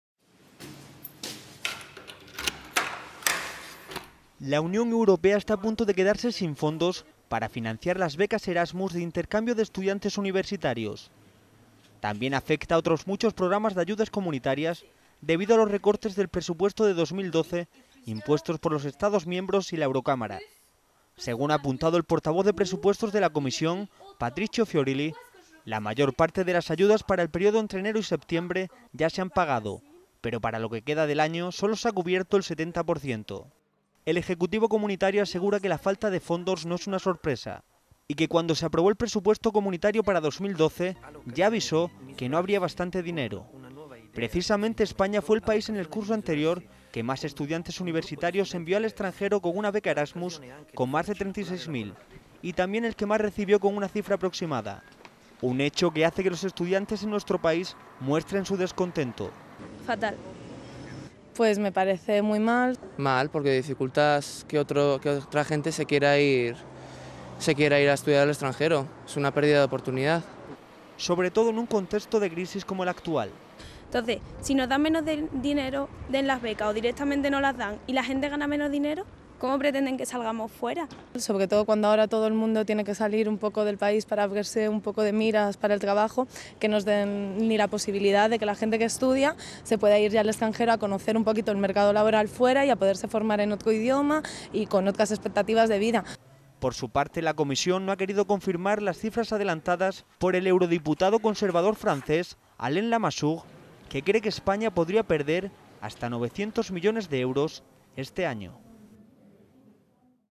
Las becas Erasmus, a punto de quedarse sin fondos
La Unión Europea está a punto de quedarse sin fondos para financiar las becas Erasmus de intercambio de estudiantes universitarios y otros muchos programas de ayudas comunitarias debido a los recortes al presupuesto de 2012 impuestos por los Estados miembros y la Eurocámara.